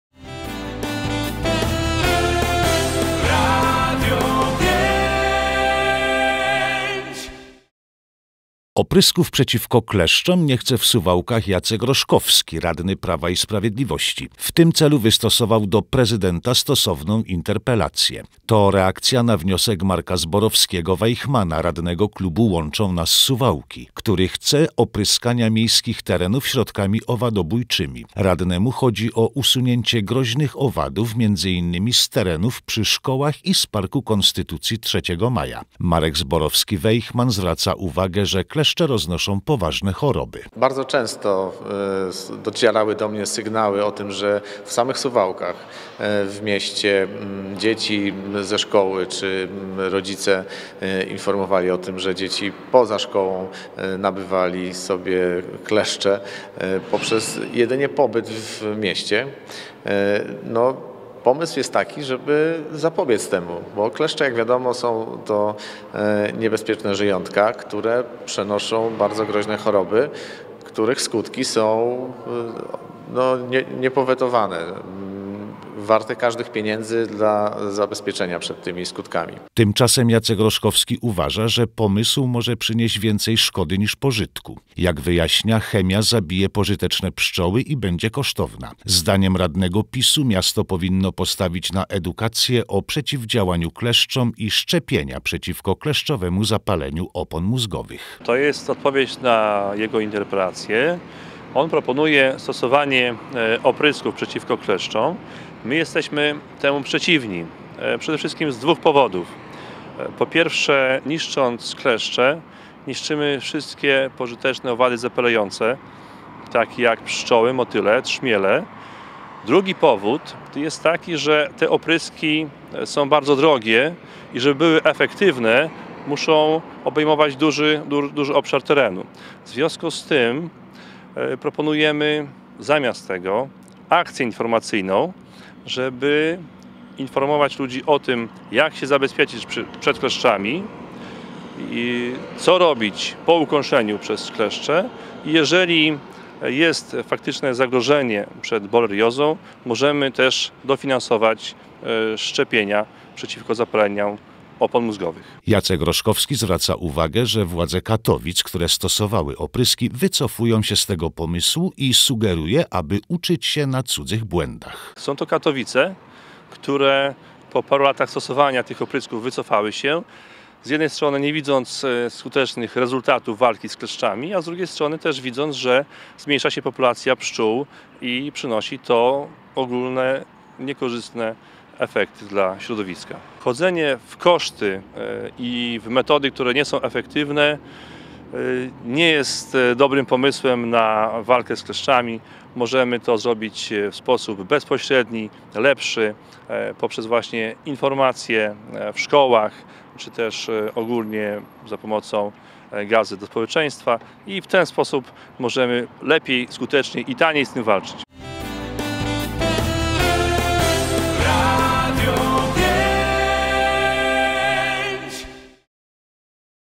Radny PiS przeciwko opryskom
Oprysków przeciwko kleszczom nie chce w Suwałkach Jacek Roszkowski, radny Prawa i Sprawiedliwości. W tym celu wystosował do prezydenta stosowną interpelację. To reakcja na wniosek Marka  Zborowskiego-Weychmana, radnego klubu "Łączą nas Suwałki", który chce opryskania miejskich terenów środkami owadobójczymi. Radnemu chodzi o usunięcie groźnych owadów między innymi z terenów przy szkołach i z Parku Konstytucji 3 Maja. Marek Zborowski-Weychman zwraca uwagę, że kleszcze roznoszą poważne choroby.
Tymczasem Jacek Roszkowski uważa, że pomysł może przynieść więcej szkody niż pożytku. Jak wyjaśnia, chemia zabije pożyteczne pszczoły i będzie kosztowna. Zdaniem radnego PiSu miasto powinno postawić na edukację o przeciwdziałaniu kleszczom i szczepienia przeciwko kleszczowemu zapaleniu opon mózgowych.
Jacek Roszkowski zwraca uwagę, że władze Katowic, które stosowały opryski, wycofują się z tego pomysłu i sugeruje, aby uczyć się na cudzych błędach.